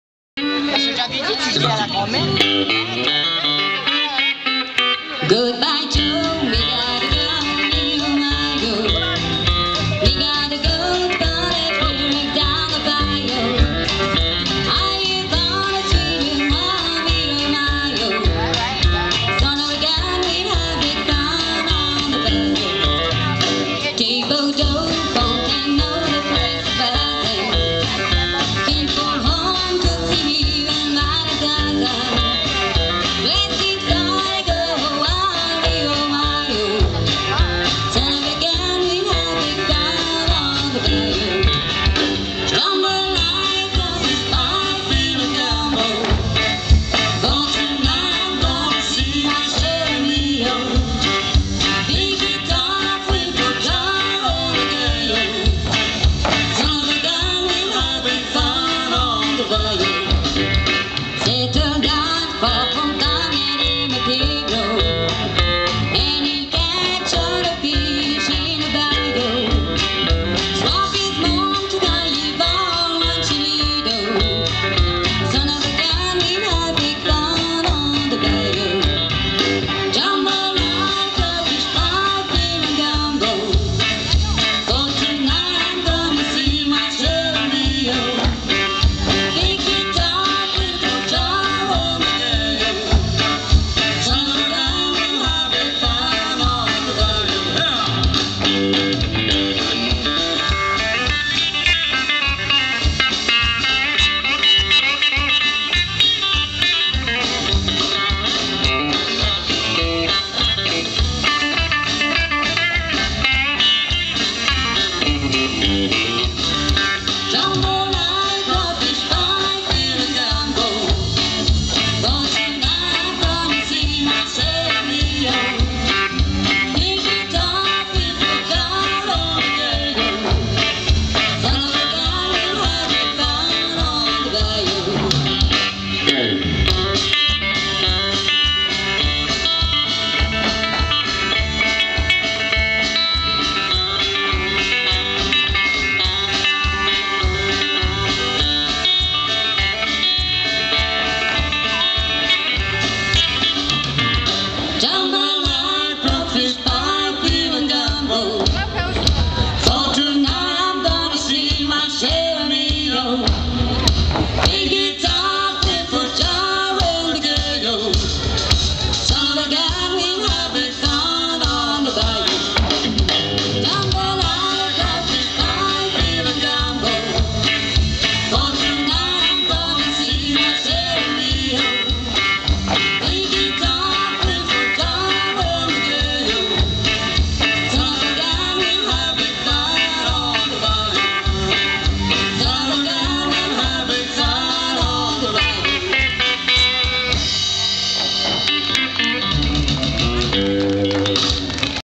Petite danse country aux Festiv'Uckange
Petite danse country aux Festiv'Uckange avec le groupe Last Gust pour la musique et les danseurs du groupe Memphis